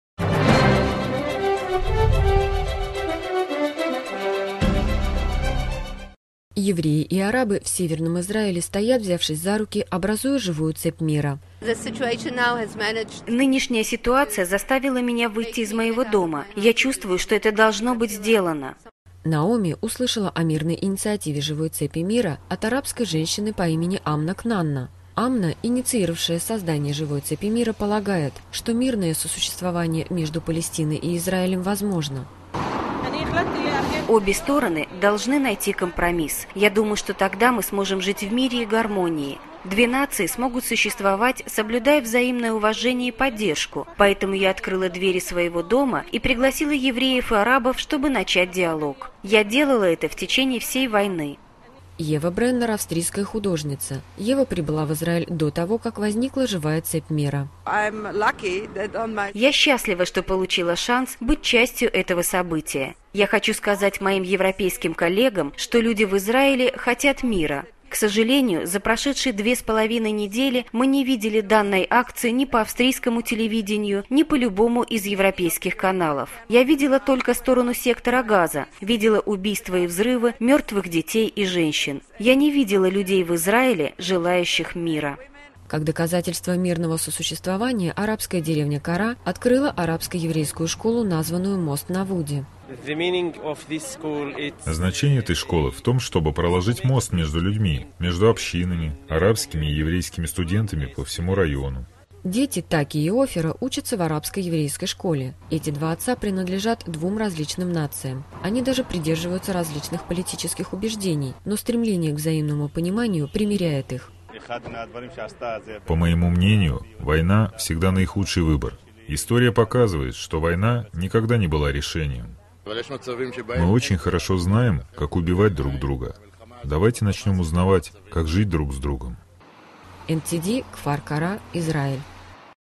В Израиле взялись за руки ради мира
Полагая, что залогом реального мира является взаимное уважение, арабы и евреи в Северном Израиле взялись за руки, демонстрируя, что мир реален.